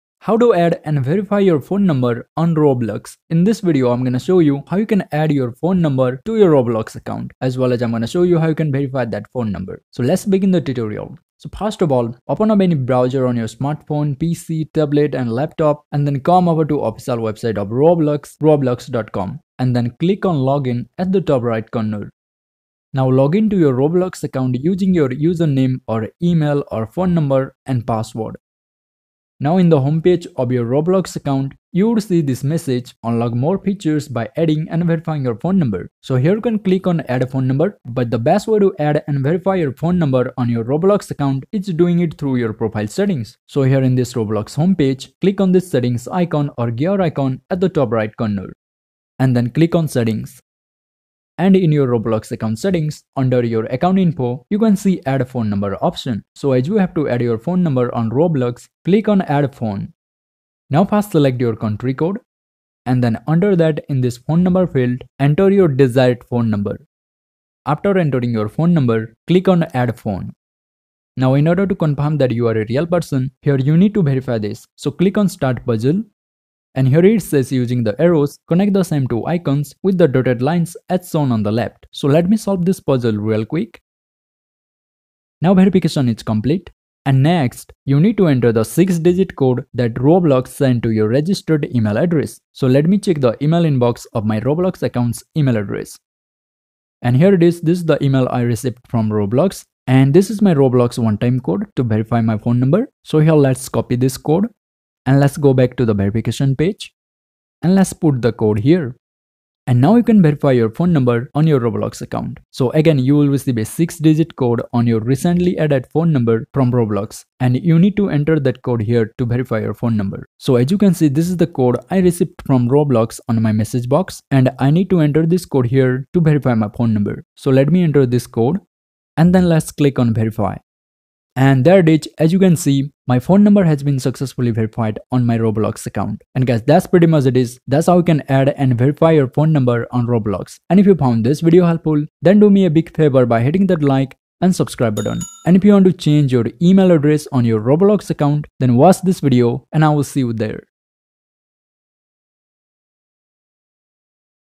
How To Add & Verify Your Phone Number On Roblox!
How to add phone number to Roblox account. How to add and verify phone number on Roblox.

In this video I'll show you how you can add or link your mobile phone number on your Roblox account and verify that. So If you don't know how to add and verify your roblox phone number, then this video is going to be very helpful for you. So make sure you watch this video all the way to end and you can add and verify phone number on Roblox account easily.